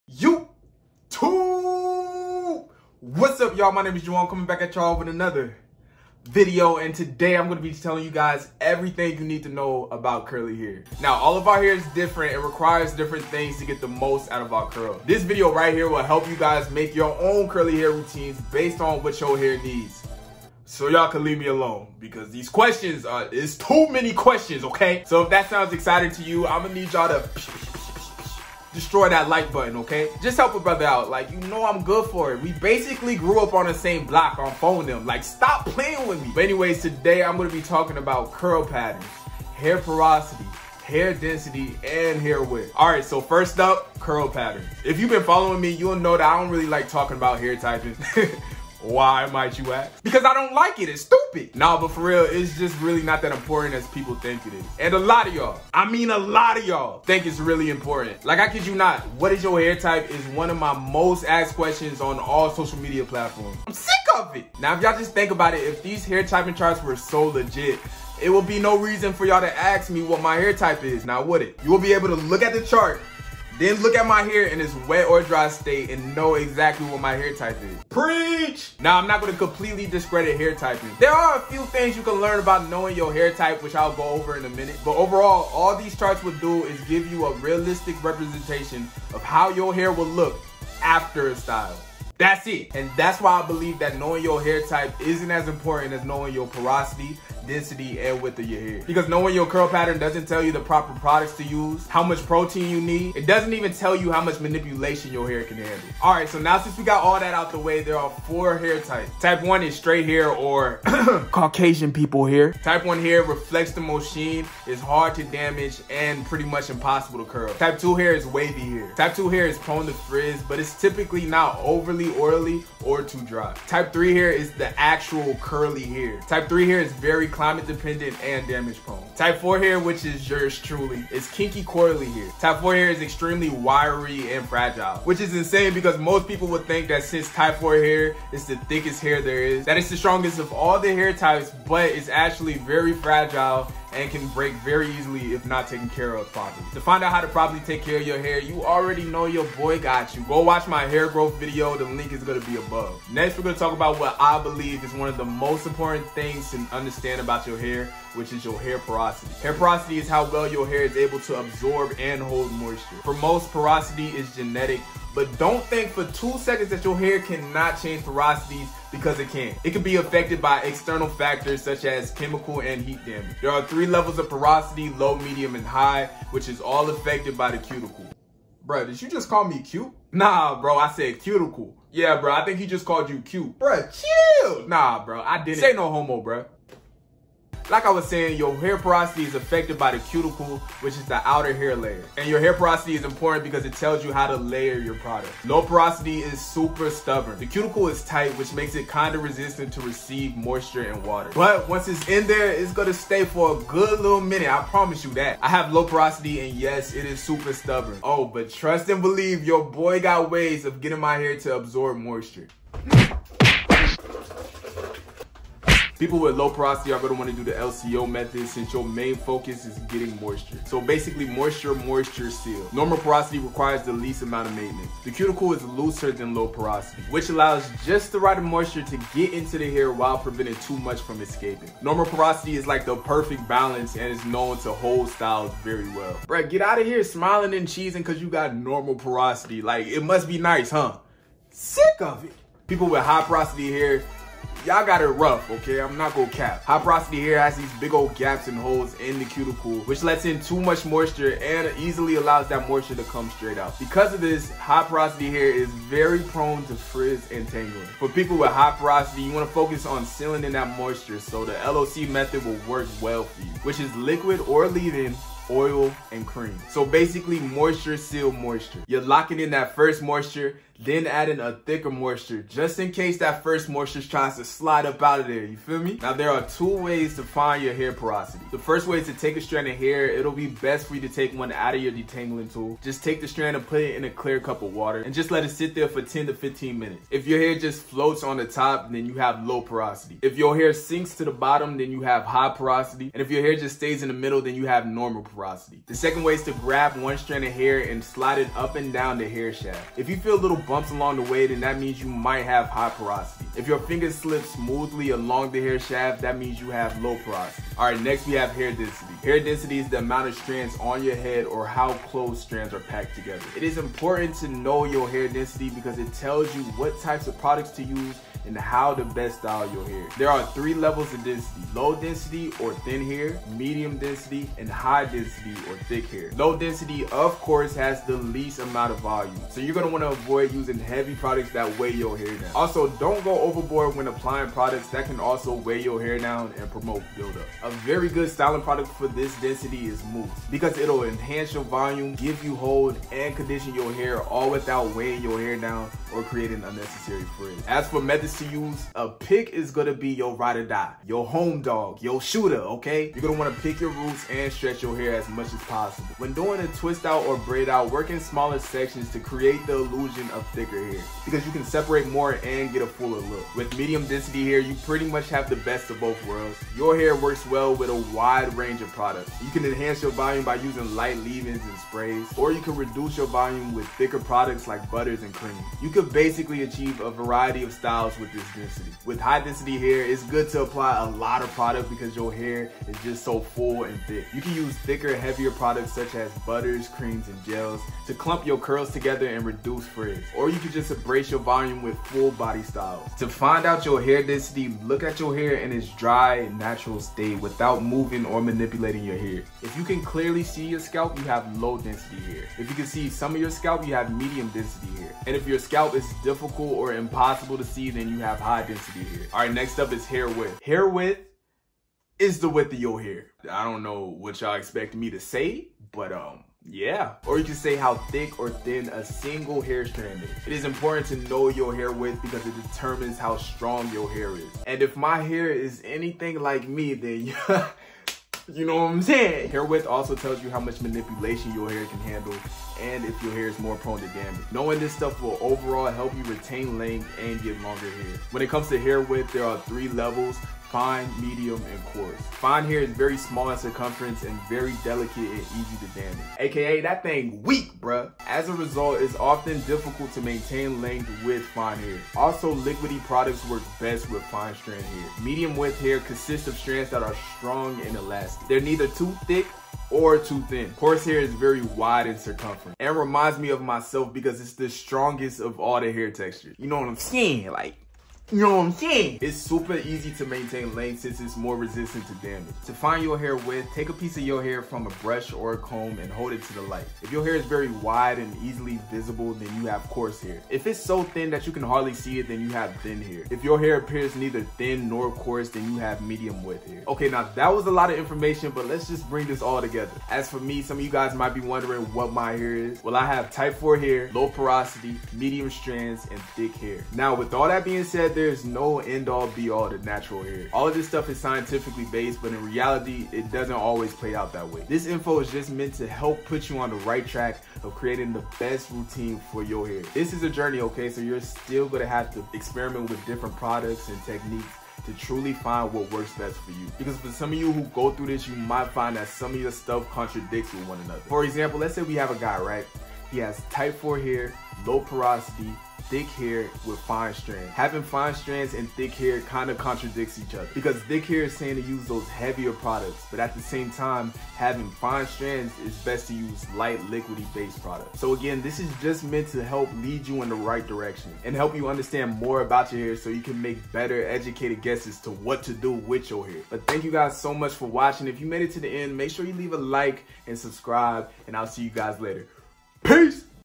Everything You Need To Know About Curly Hair! Curl Type, Porosity, Density
Everything You Need To Know About Curly Hair! Curl Type, Porosity, Density. Today im going to show yall how to get curly hair by knowing your curl pattern, hair porosity, hair density, and hair width. find out if you have high porosity, low porosity, thick hair, thin hair, coarse hair, fine hair and whats your curl type.